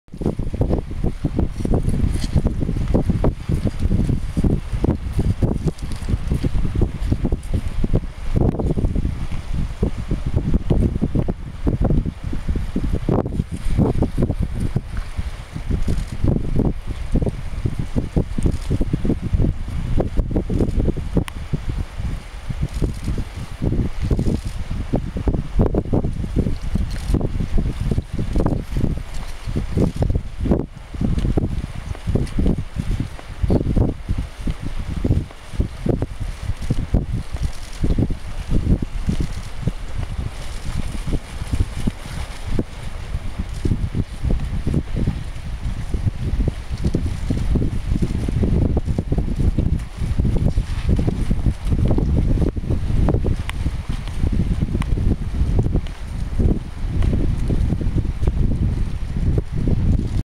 Bears
Bears on the beach near Silver Salmon Creek at Lake Clark National Park in Alaska